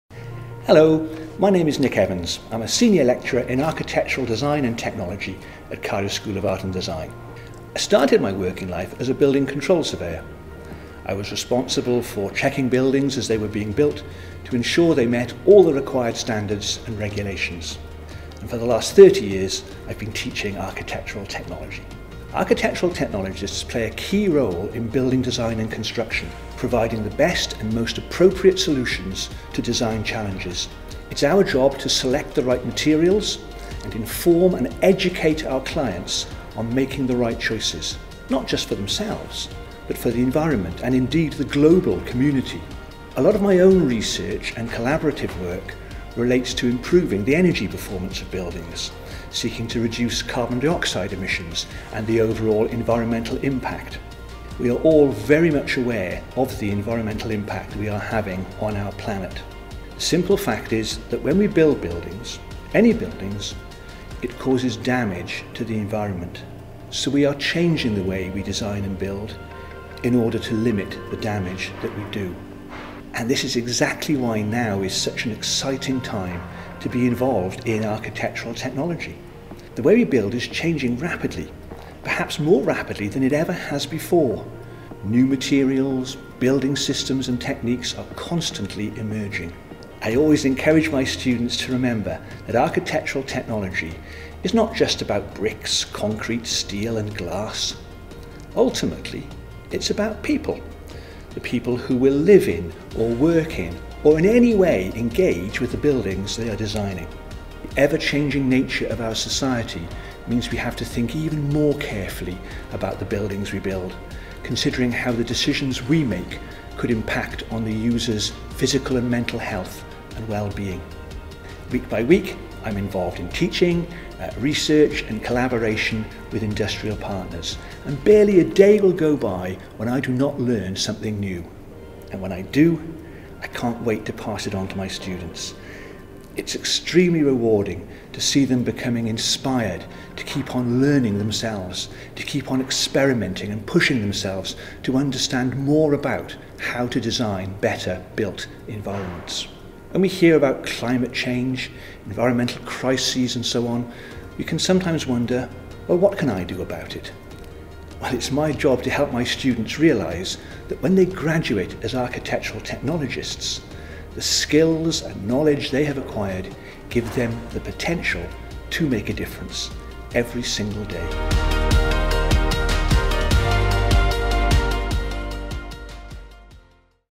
Meet the Team – Nick Evans | BSc (Hons) Architectural Design and Technology | Cardiff Met
"It’s my job to help my students realise that when they graduate as architectural technologists, the skills and knowledge they have acquired give them the potential to make a difference every single day”

Meet Nick Evans, Senior Lecturer in Architectural Design and Technology at Cardiff Metropolitan University.